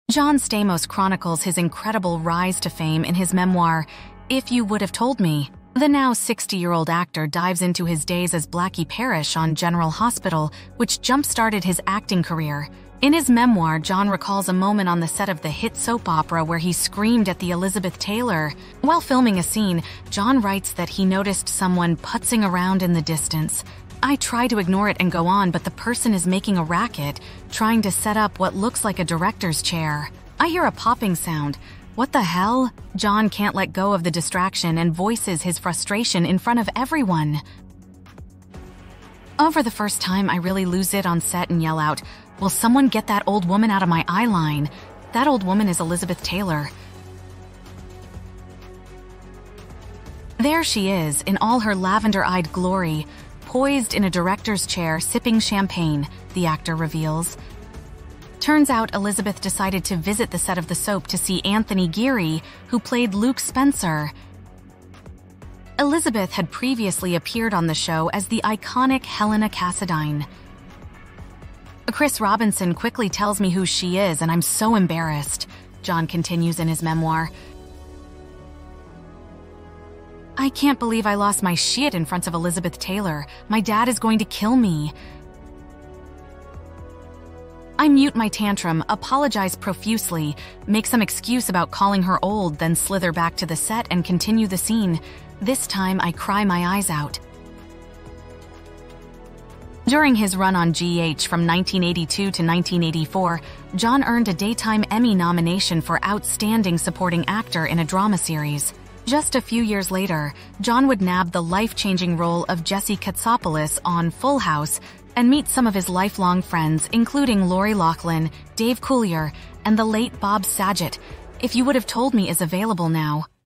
How John Stamos Disrespected Elizabeth Taylor
John Stamos, the beloved actor known for his roles on iconic TV shows like "Full House" and "ER," recently shared an amusing and somewhat cringe-worthy story from his early acting days on the set of "General Hospital."
In a candid and humorous revelation, Stamos admitted to a major slip-up during his time working alongside the legendary Elizabeth Taylor on the popular soap opera "General Hospital." According to Stamos, he mistakenly referred to Taylor as "old" during a conversation, which left him utterly embarrassed.
The incident offers a charming insight into the world of showbiz and the awkward moments that even famous actors can find themselves in. Stamos, who has been a heartthrob in the entertainment industry for years, was able to laugh at his own youthful misstep.
This anecdote underscores the timeless allure and appeal of celebrities like Elizabeth Taylor and the lessons learned by rising stars like John Stamos as they navigate the complexities of fame and stardom.
Stamos' willingness to share this amusing story reveals his humility and authenticity, making him all the more endearing to fans. It's a reminder that even in the presence of Hollywood royalty, everyone has their moments of embarrassment and awkwardness.
As John Stamos continues to entertain audiences with his talent and charisma, stories like these provide fans with a deeper connection to the person behind the famous persona.